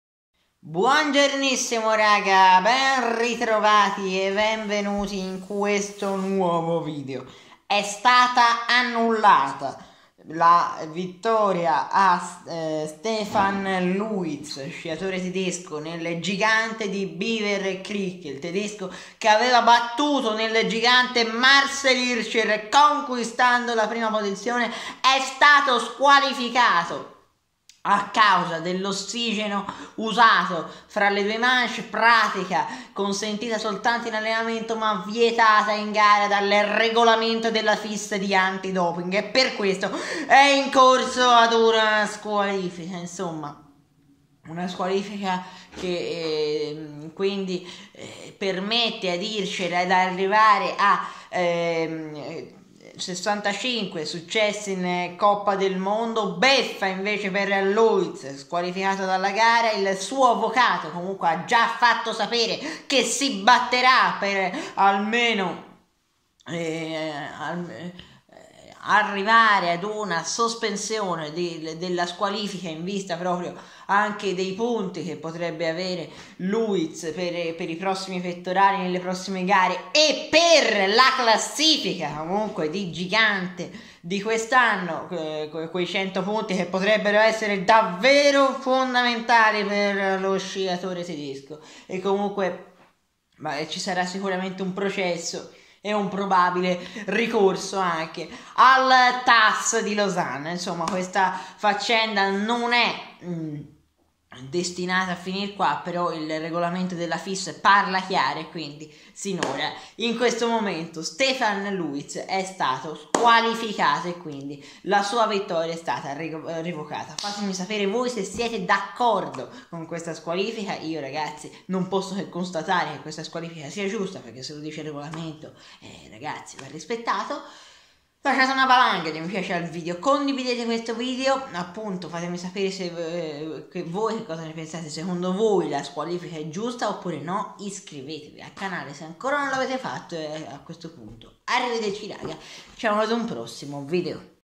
STEFAN LUITZ SQUALIFICATO PER OSSIGENO, REVOCATO IL GIGANTE DI BEAVER CREEK : VITTORIA A HIRSCHER
In virtù del regolamento FIS